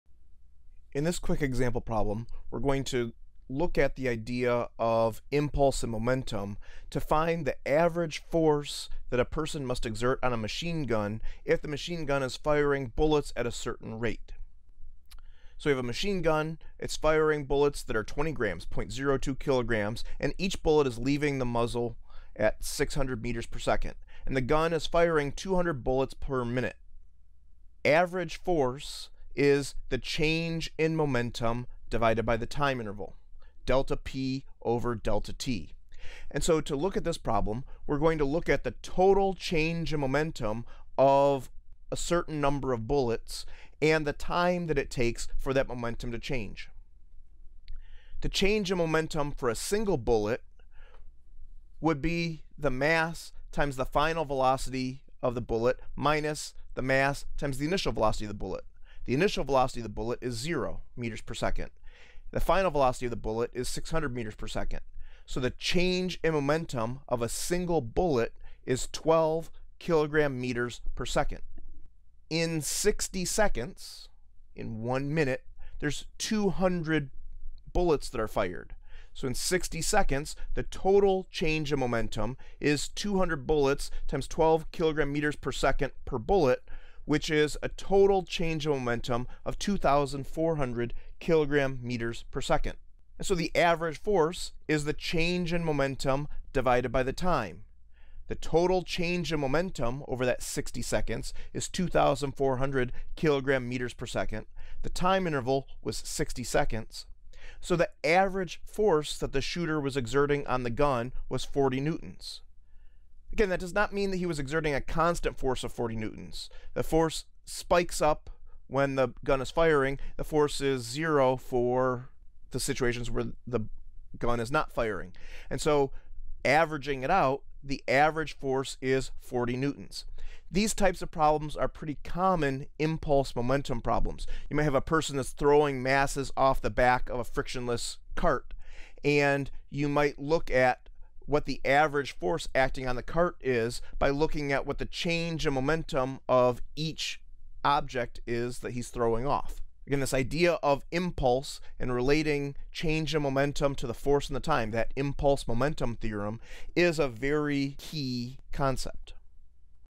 Using Impulse-Momentum Theorem to Find Average Force Acting on Machine Gun
The average force acting on a machine gun firing bullets at a given rate is calculated from looking at the change in momentum of the bullets.